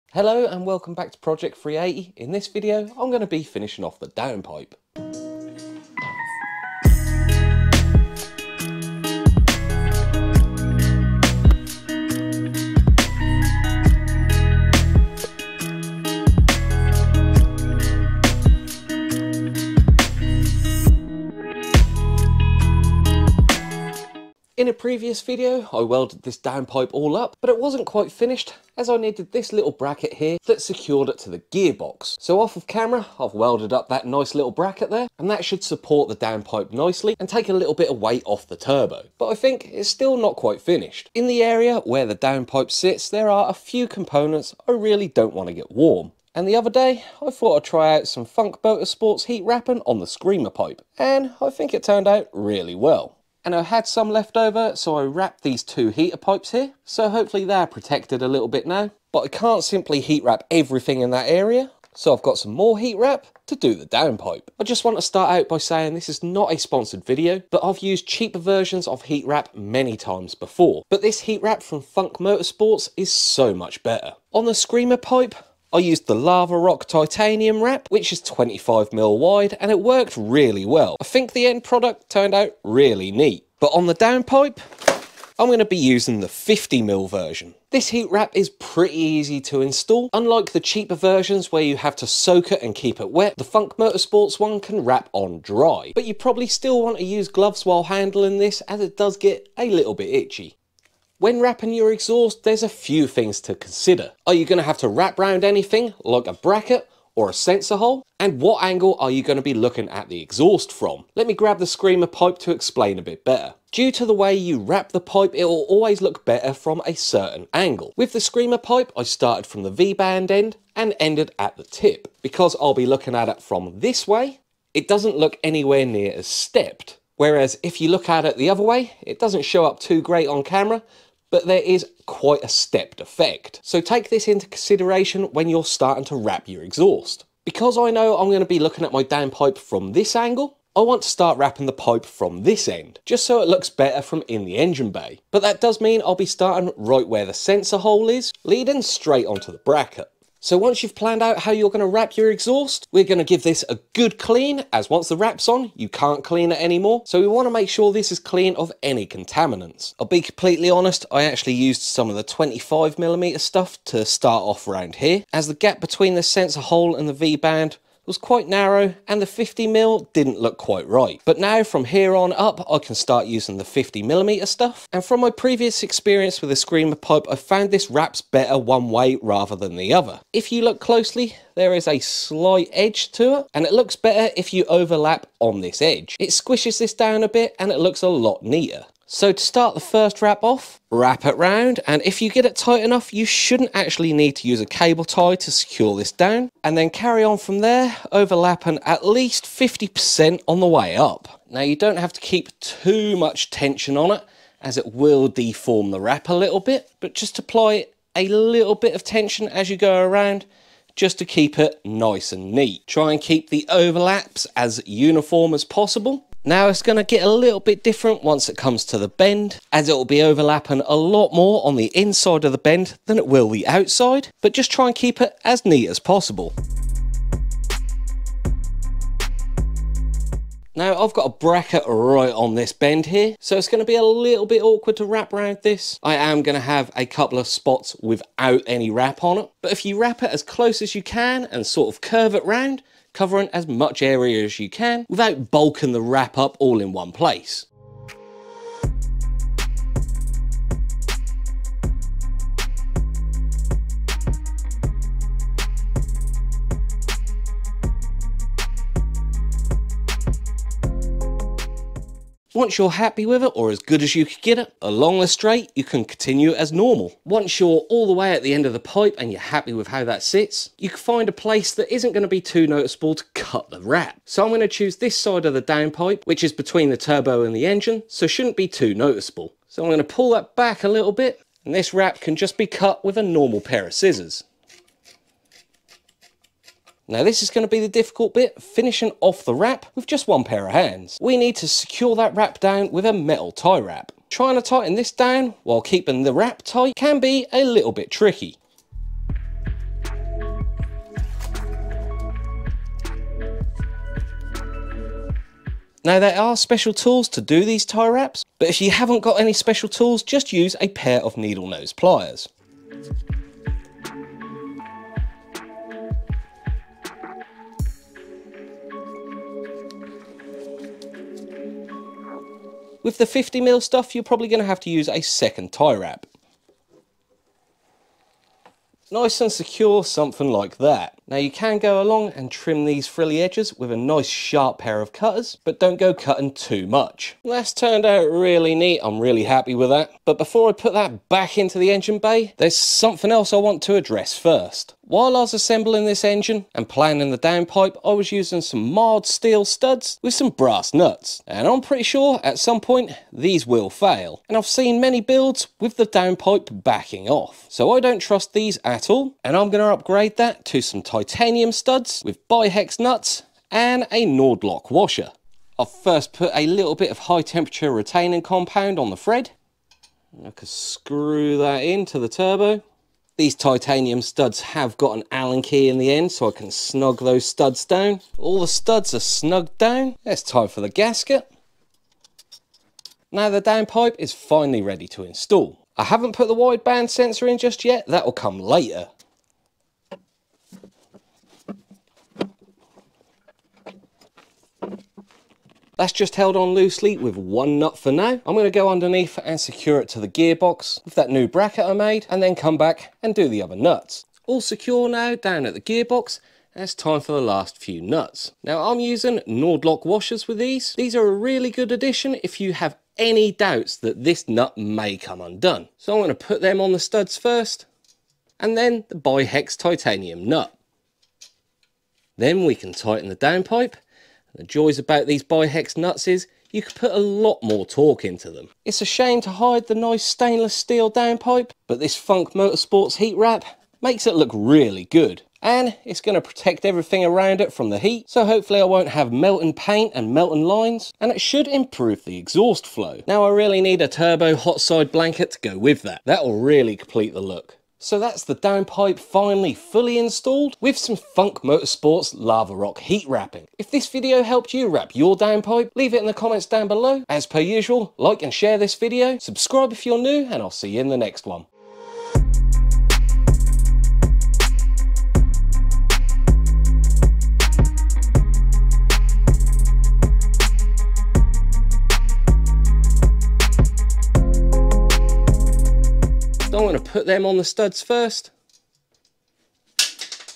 FUNK MOTORSPORTS LAVA ROCK HEAT WRAP : MX5 TURBO BUILT pt 57
Finishing the downpipe in some Funk Motorsports Lavarock titanium heat wrap, and it looks amazing